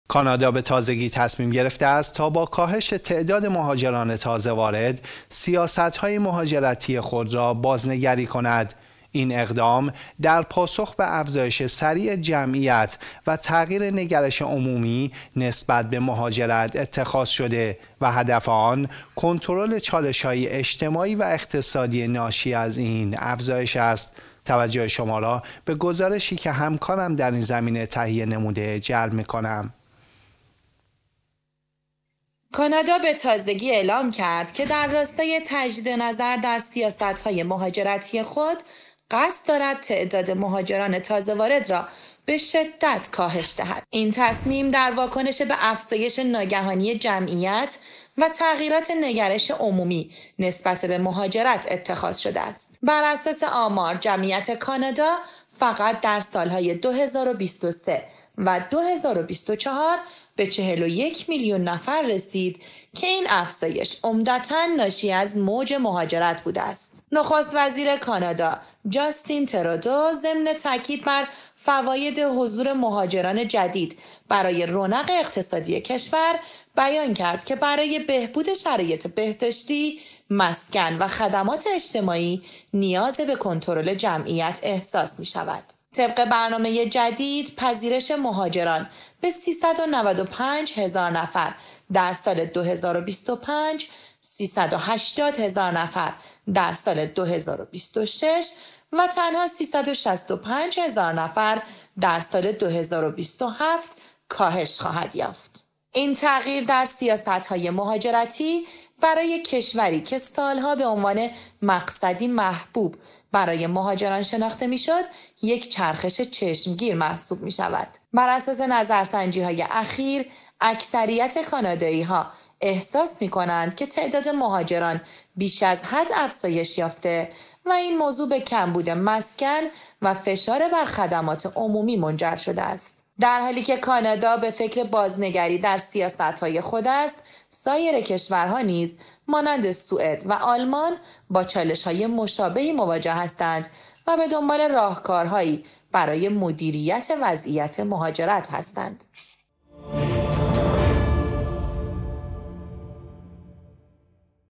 ترمز مهاجرت در کانادا: سیاست‌های جدید برای مهار افزایش جمعیت
ترمز مهاجرت در کانادا به دلیل افزایش سریع جمعیت و تغییرات نگرش عمومی، به یکی از موضوعات داغ تبدیل شده است. کانادا به تازگی با اعلام سیاست‌های جدید مهاجرتی خود، قصد دارد تعداد مهاجران تازه‌وارد را به شدت کاهش دهد. این تغییرات برای مقابله با بحران مسکن، خدمات بهداشتی و اجتماعی است. در این ویدیو به بررسی جزئیات این تصمیم و تاثیرات آن بر جامعه کانادا و اقتصاد می‌پردازیم. اگر به موضوعات مرتبط با مهاجرت، سیاست‌های بین‌المللی و مسائل اقتصادی علاقه دارید، این ویدیو را از دست ندهید.
📌 برای اطلاعات بیشتر و تحلیل دقیق‌تر، حتماً این ویدیو را تماشا کنید.

🛑 نظر شما در مورد این تغییرات چیست؟ در بخش نظرات با ما به اشتراک بگذارید!
👀 برای دریافت ویدیوهای بیشتر، کانال ما را دنبال کنید!